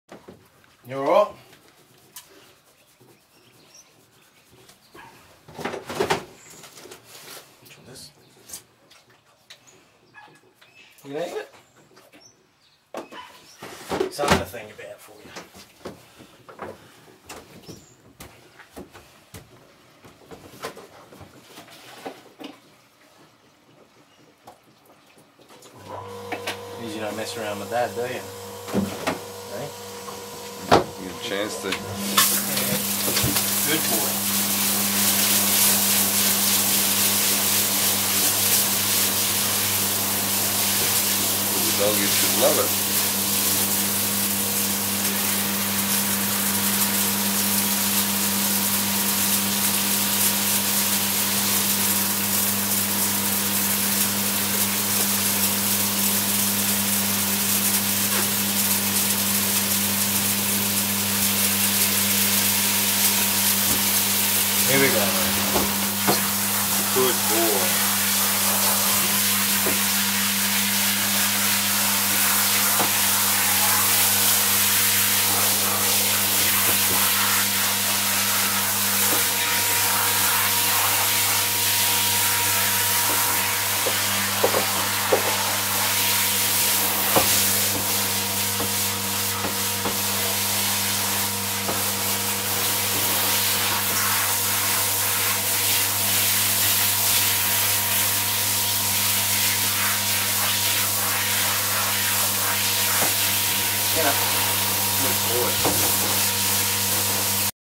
Nugget's No2 Bath
God he is such a good boy,when u can get him in the Tub